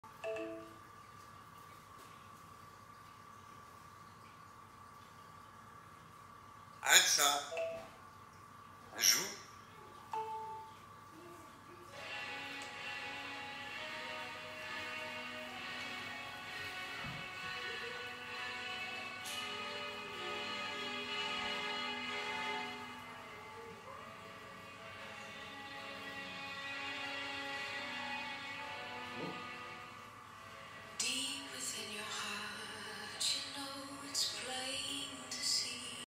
ALEXA de Amazon Music n'a plus de secret pour Elvis
Ca y est ! Elvis est HI-TECH ! Et... il a pris le contrôle de la musique à la maison !